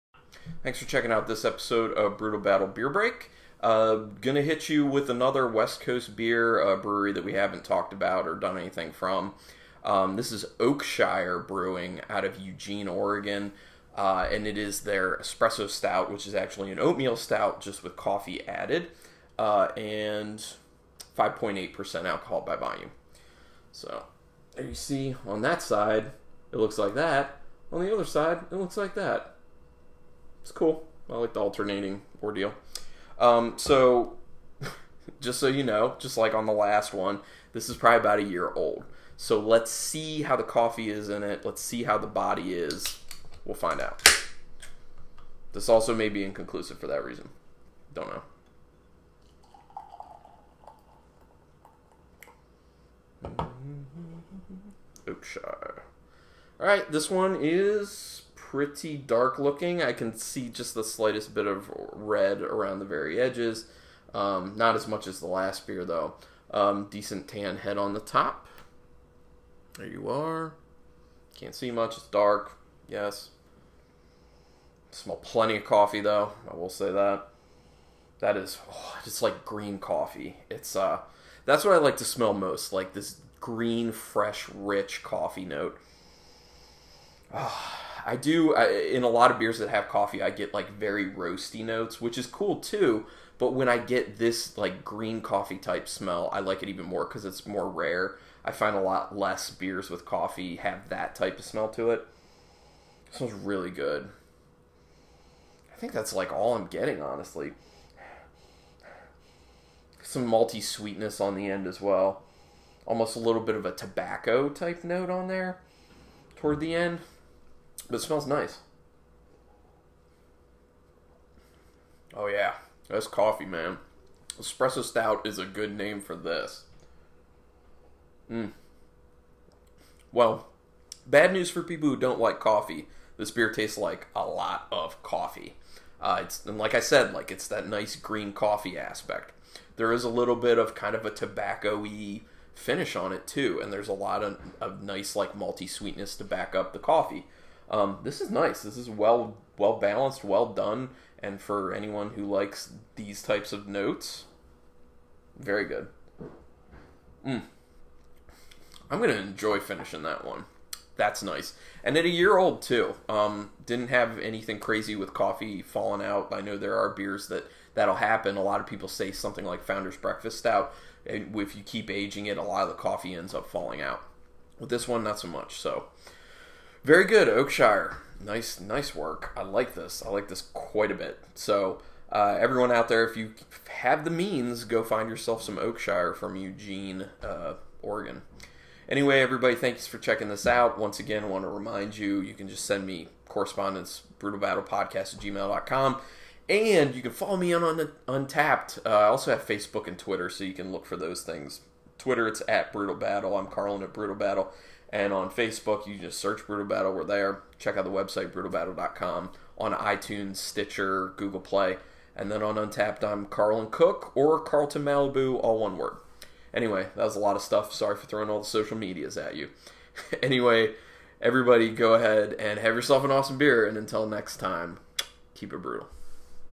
Brewtal Battle Beer Break - Espresso Stout by Oakshire Brewing (Ep. 111)
Carlin tastes Espresso Stout Oatmeal Stout by Oakshire Brewing Company out of Eugene, Oregon.